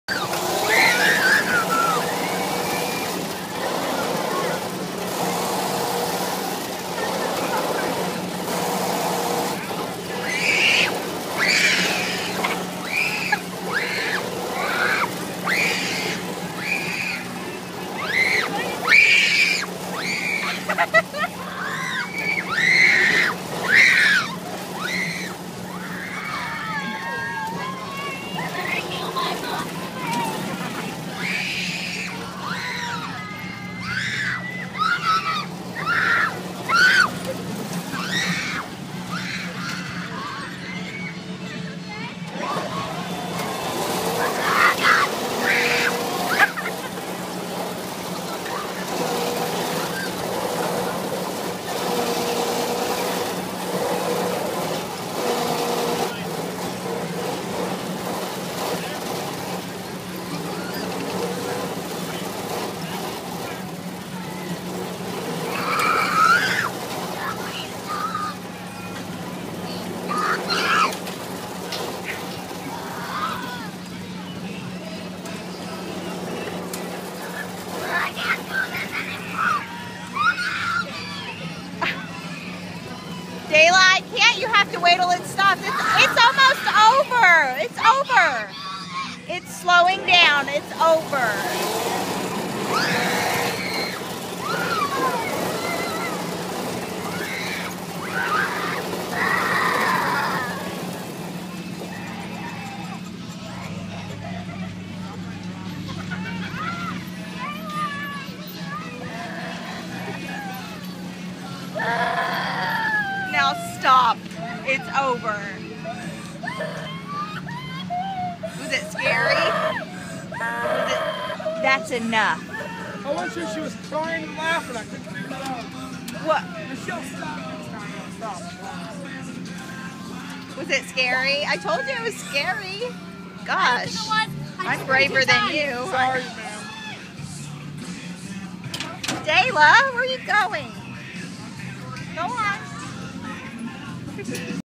Dayla's first scary carnival ride